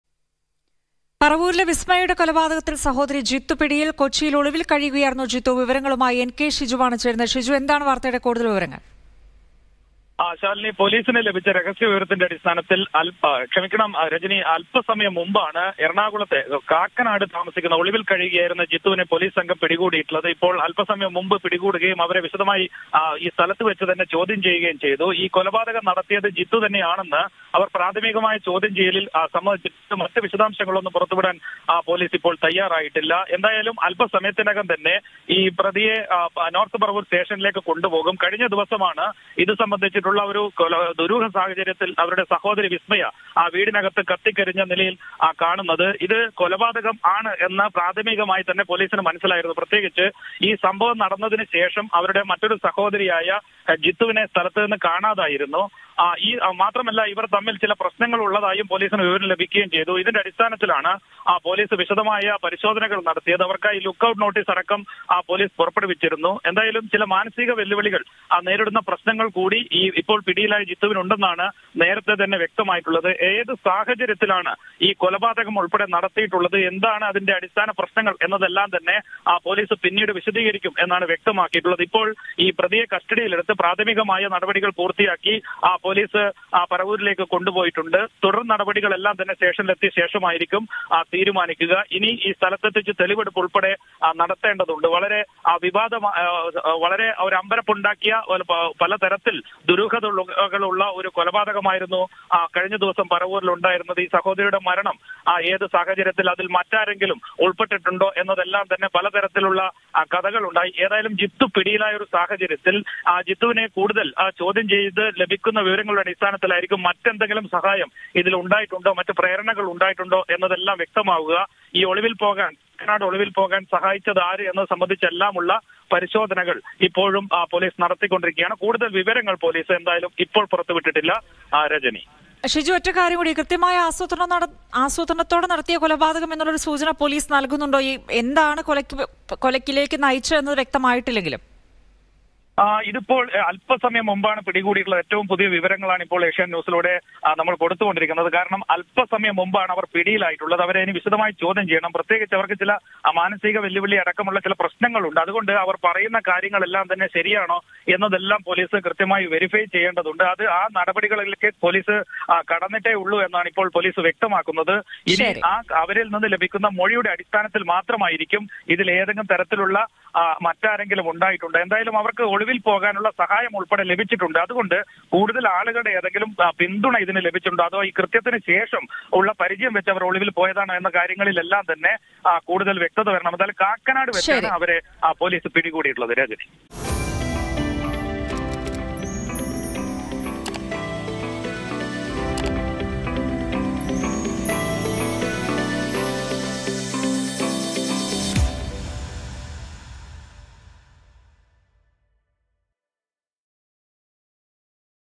പറവൂരിലെ വിസ്മയയുടെ കൊലപാതകത്തില്‍ സഹോദരി പിടിയില്‍ | Paravur Vismaya Murder Case
പറവൂരിലെ വിസ്മയയുടെ കൊലപാതകത്തില്‍ സഹോദരി ജിത്തു പിടിയില്‍.
Paravur Vismaya Death Case: Accused  Jithu arrested.